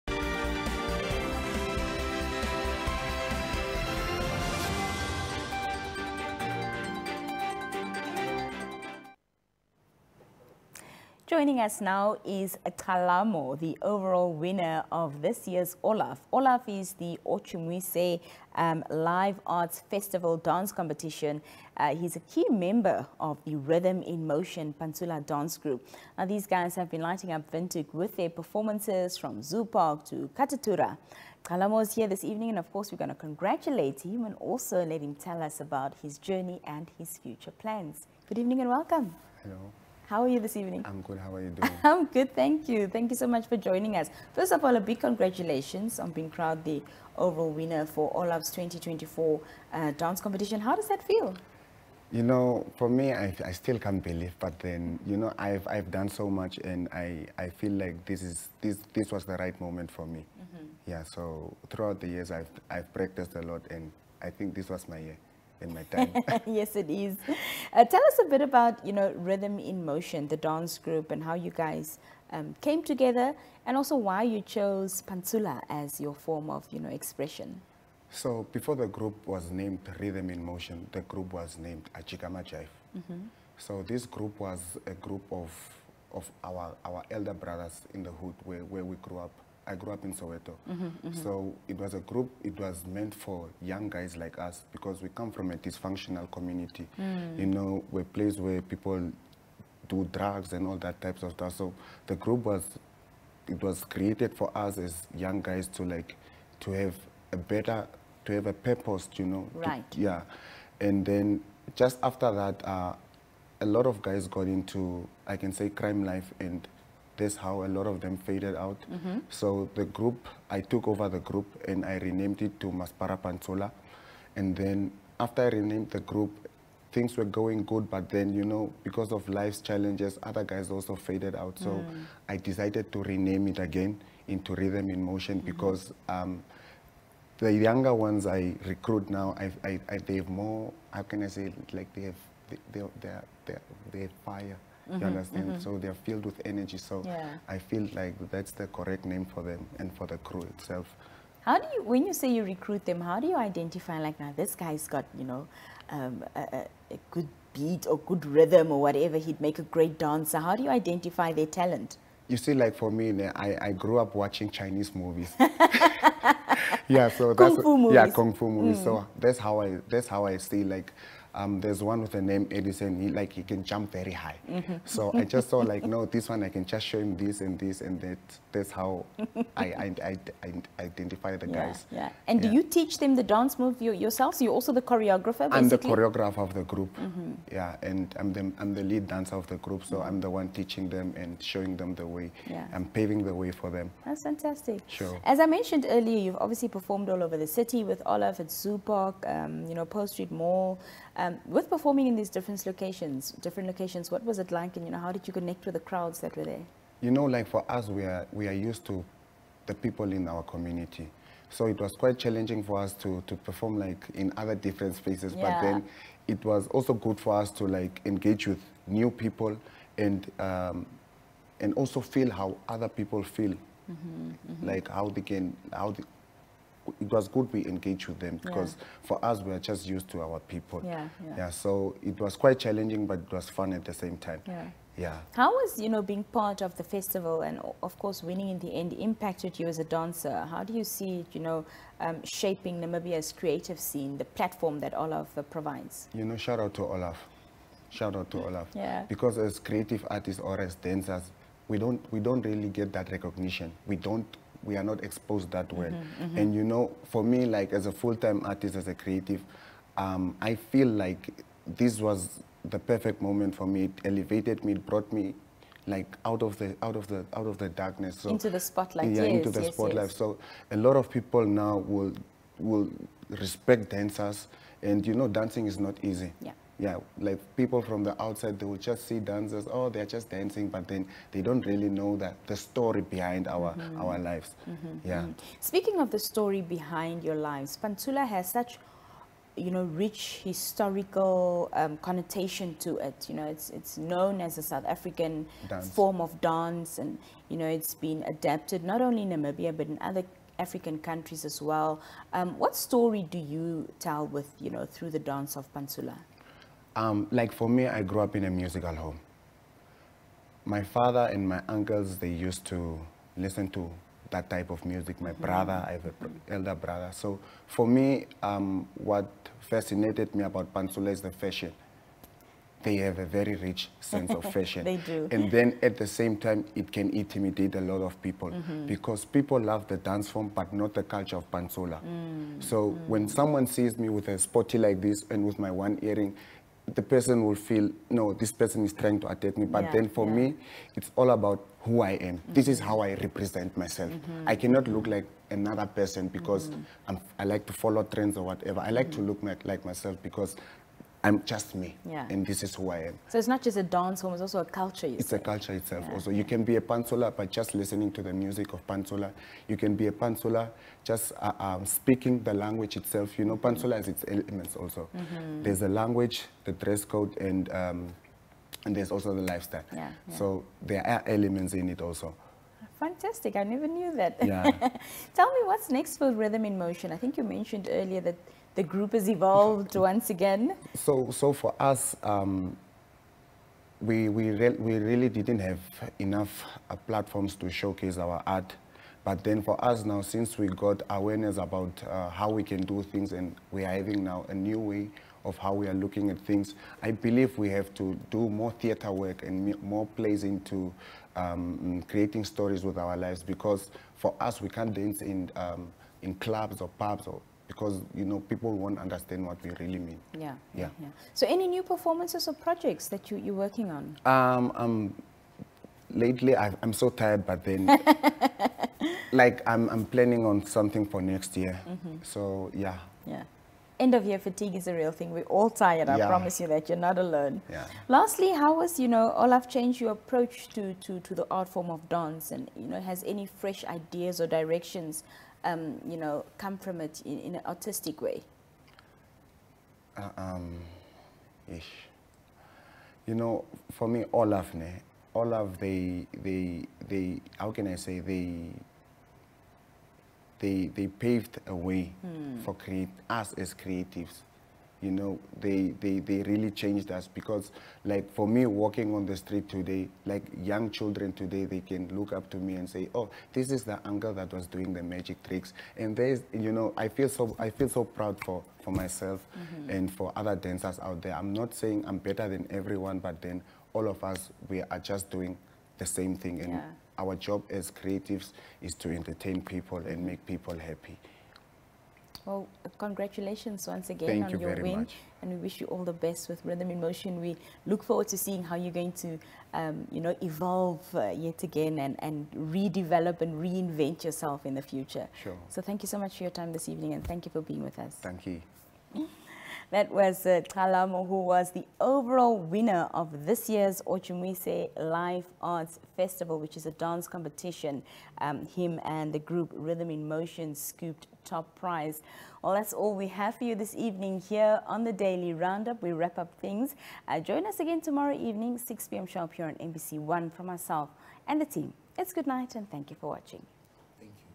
THE DAILY ROUNDUP WITH NINA | Xallamo Shines as Winner of OLAF Dance Competition - nbc
The overall winner of this year's OLAF dance competition and a standout member of the Rhythm in Motion Pantsula Dance Group is Xallamo.

They've been electrifying Windhoek with their incredible performances, bringing life and energy to venues from Zoo Park to Katutura.